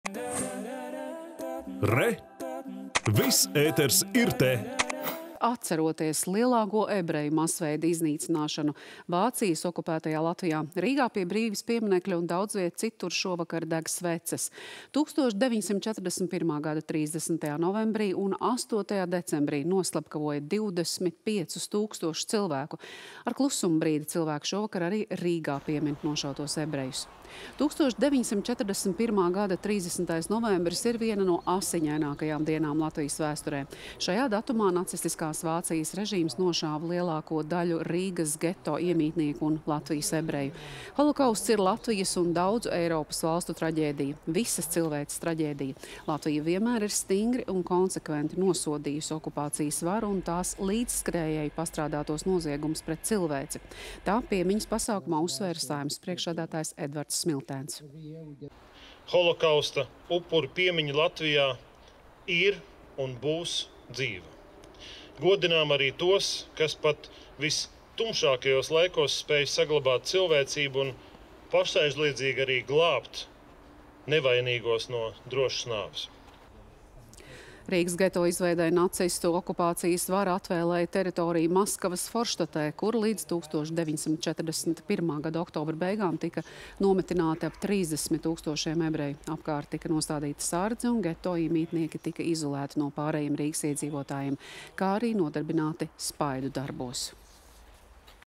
Piemin Rumbulas traģēdiju
Atceroties lielāko ebreju masveida iznīcināšanu Vācijas okupētajā Latvijā, Rīgā pie Brīvības pieminekļa un daudzviet citur šovakar deg sveces. 1941. gada 30. novembrī un 8. decembrī   noslepkavoja 25 000 cilvēku. Ar klusuma brīdi  cilvēki šovakar arī Rīgā piemin nošautos ebrejus.